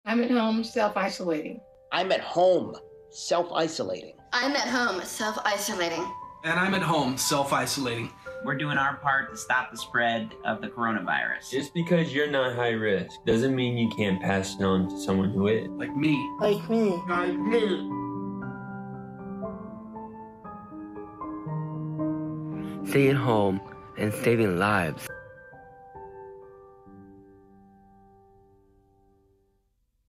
We're in This Together PSA#3 30s
An important message about COVID-19 from some incredible, multi-talented friends within the disability community. They remind us that we are all in this together, so please self-isolate, stay at home, and do your part to stop the spread of this virus – especially because people with disabilities may be particularly vulnerable. Thank you to filmmaker extraordinaire John Lawson for quickly spearheading this urgent public service project! Fun fact, everyone you see in this PSA are active participants (and super spokespeople) for the Easterseals Disability Film Challenge. Together, we are a mighty group! Featuring Danny Woodburn, Jamie Brewer, Tobias Forrest, Nic Novicki, Diana Elizabeth Jordan, Micah Fowler, Cole Sibus